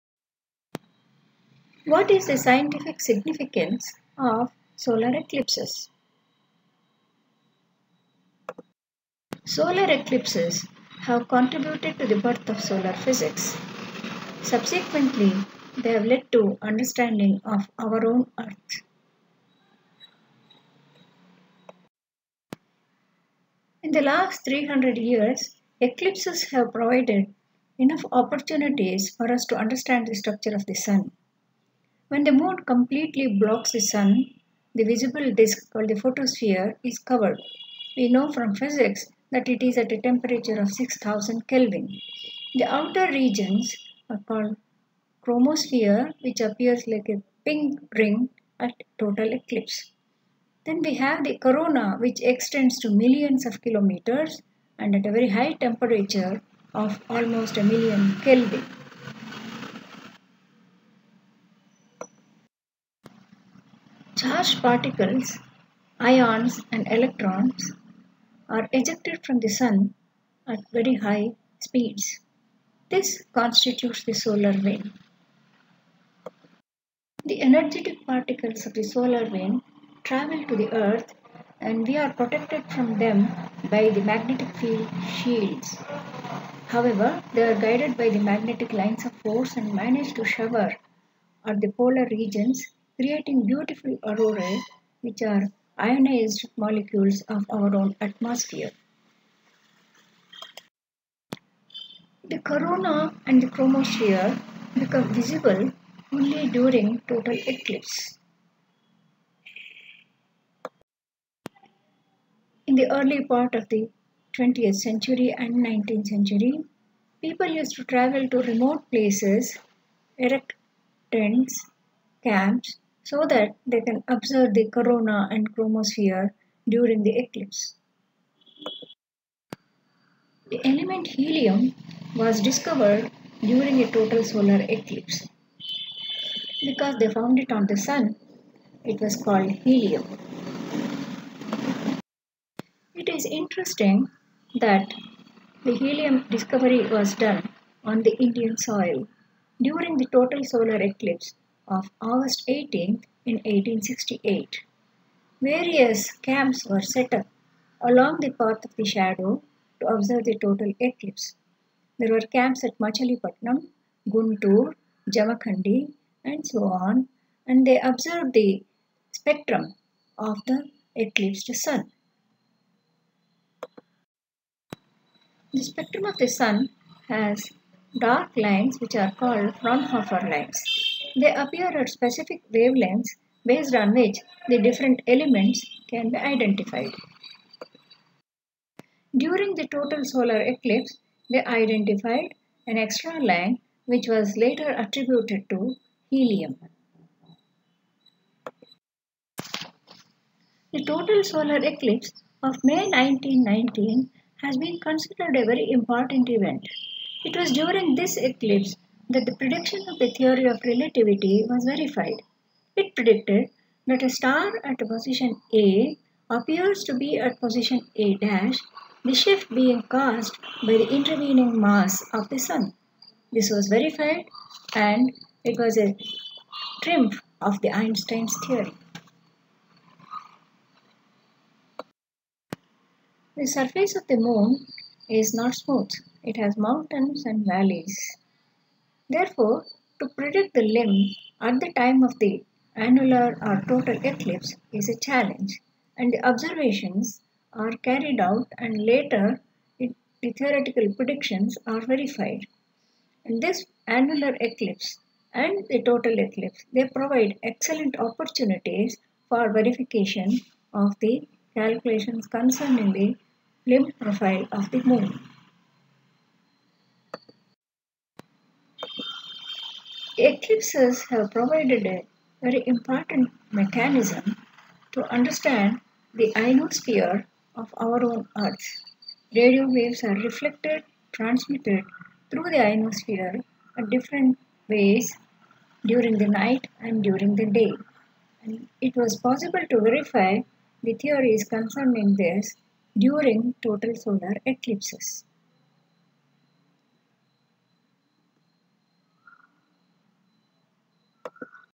Science from eclipses
This video explains the importance of solar eclipses in understanding the structure of the sun and the basics of sun-earth interactions.
This was based on an answer to a question asked by Smt Usha Narayanan in the phone in session - the Kannada version is available here